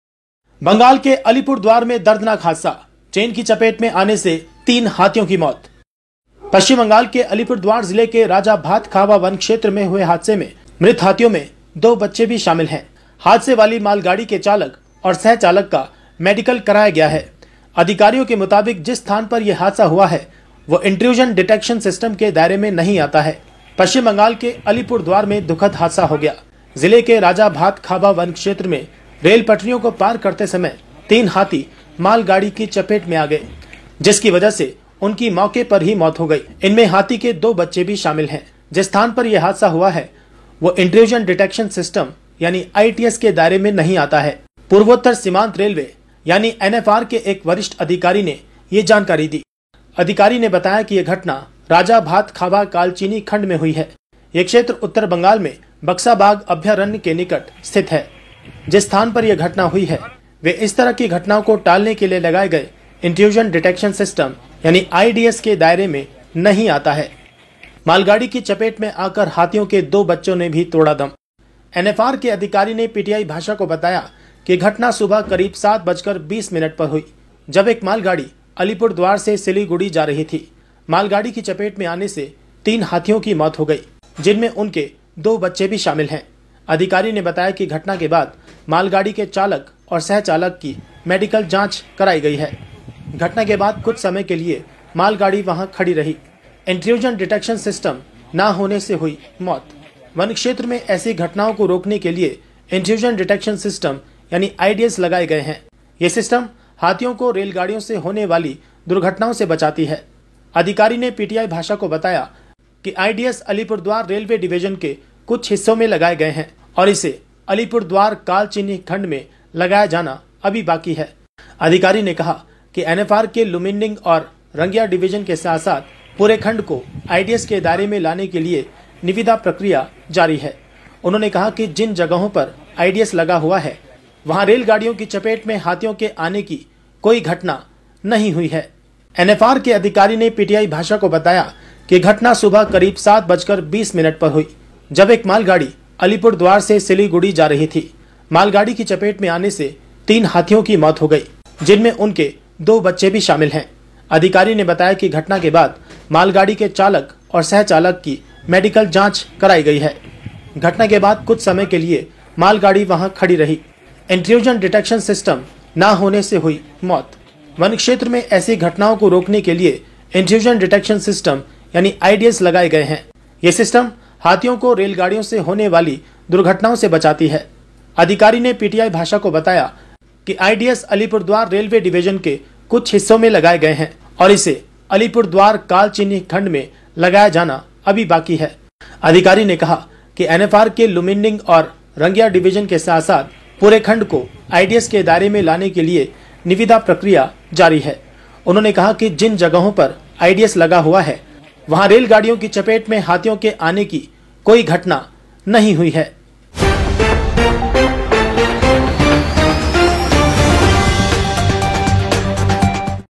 3 Elephants Die After Being Hit By Train In Horrifying Bengal Accident
3 Elephants Die After Being Hit By Train In Horrifying Bengal Accident